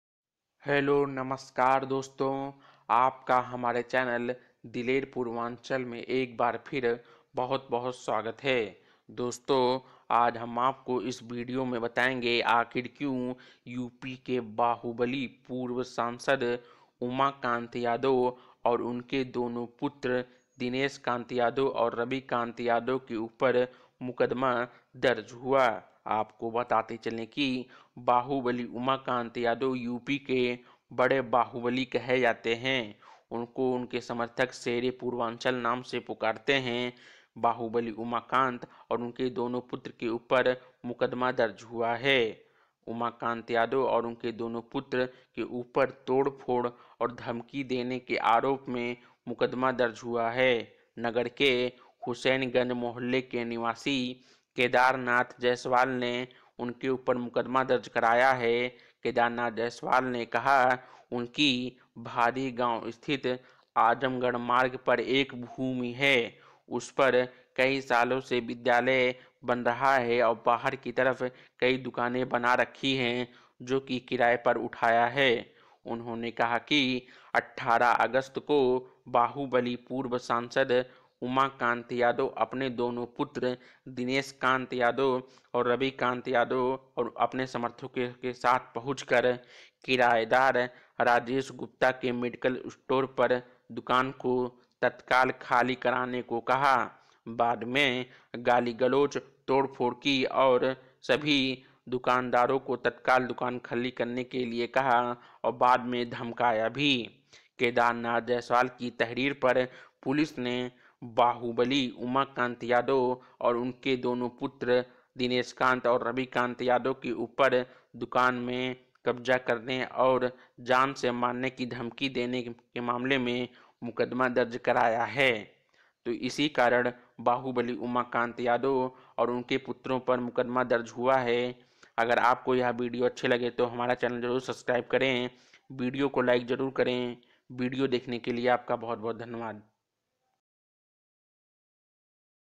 बाहुबली उमाकांत यादव और उनके बेटो के खिलाफ क्यों दर्ज हुआ मुकदमा । पूर्व सांसद उमाकांत यादव
बाहुबली उमाकांत यादव और उनके बेटे दिनेशकांत रविकांत के खिलाफ क्यों मुक़दमा दर्ज हुआ जाने